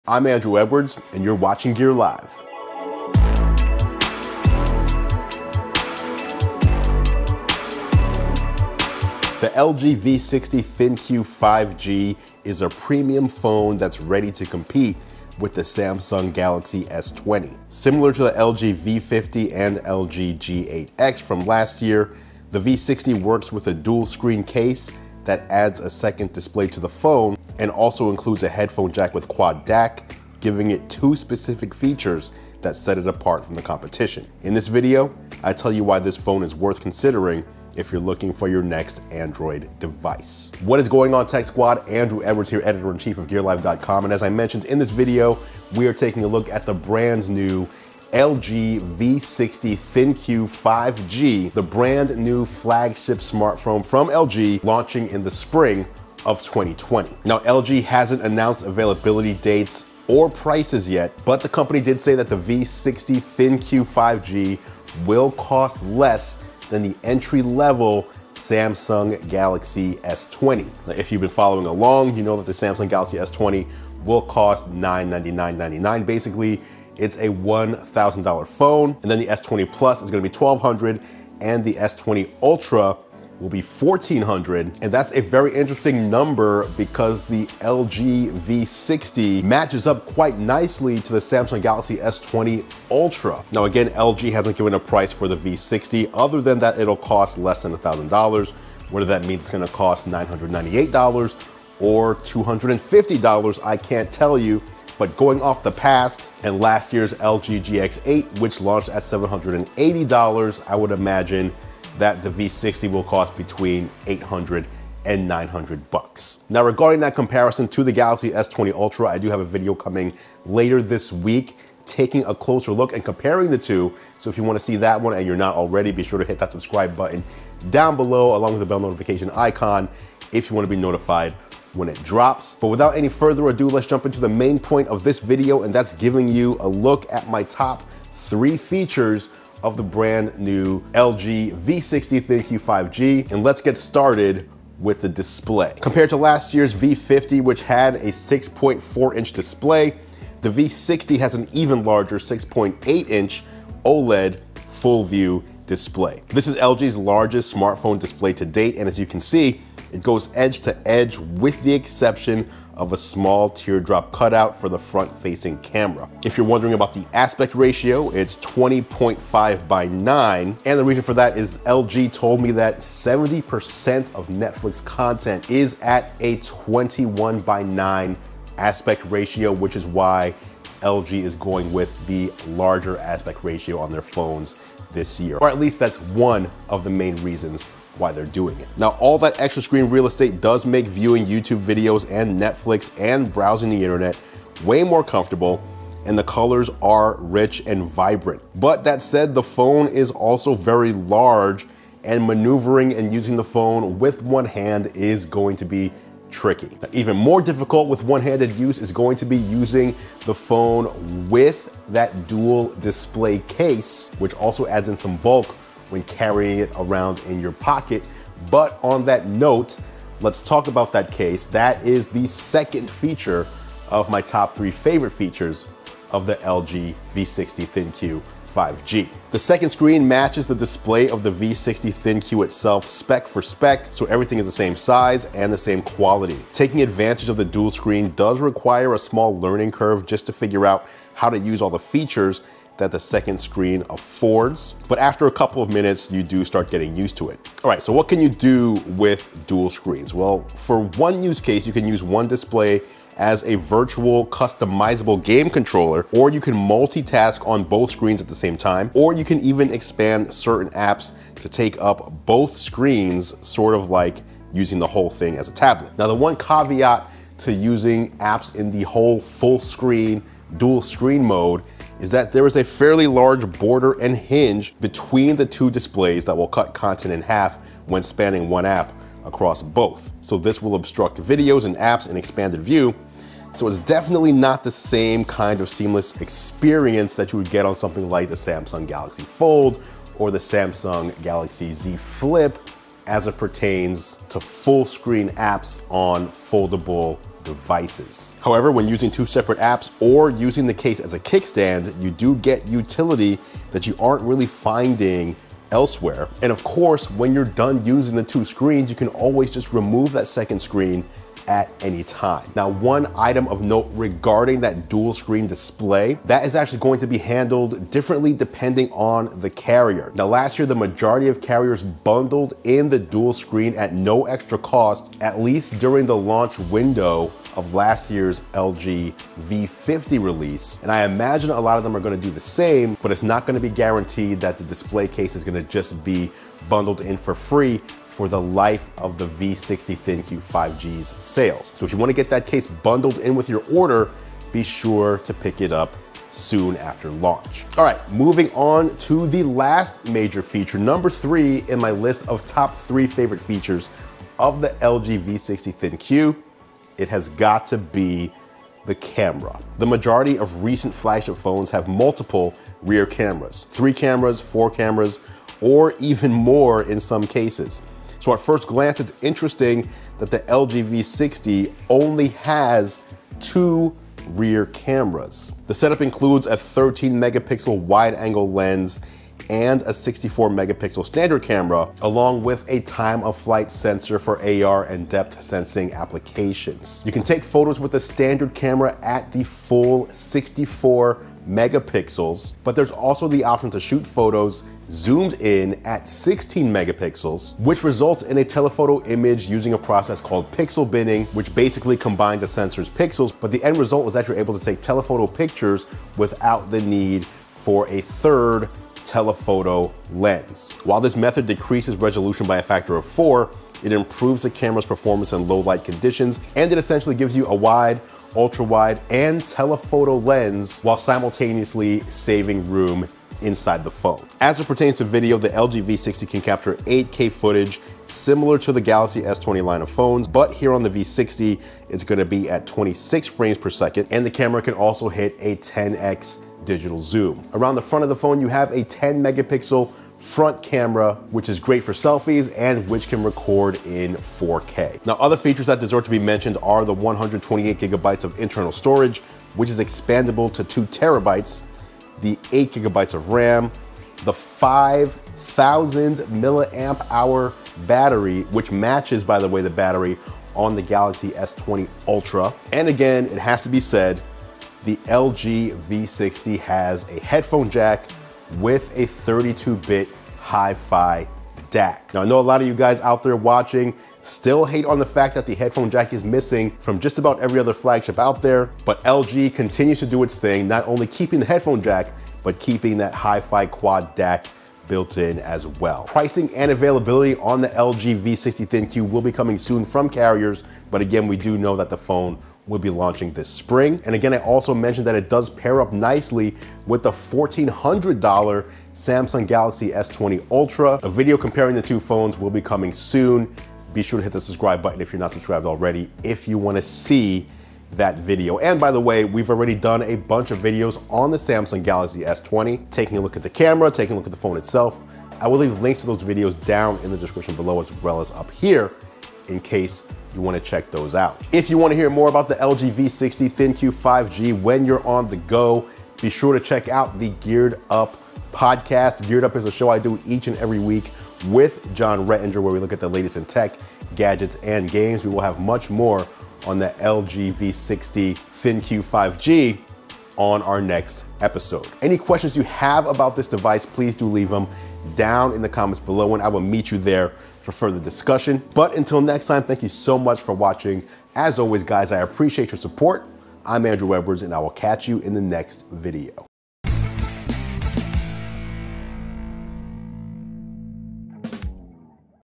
LG V60 ThinQ 5G Hands-on Impressions: Top 3 Features!
In this video I bring you a first-look at the LG V60 ThinQ 5G - the next flagship smartphone from LG, featuring a dual-display case and a host of other features. The best LG V60 5G features are the focus here, looking at the OLED display, the dual display case, and the LG V60 camera which is impressive in its own right.

This is a great device and the LG V60 ThinQ vs Samsung Galaxy S20 is going to be a real fight when looking at the best Android smartphones 2020 has to offer.

camera: Panasonic Lumix S1H
audio: Sennheiser MKE600 Shotgun Mic
computer: iMac Pro

MUSIC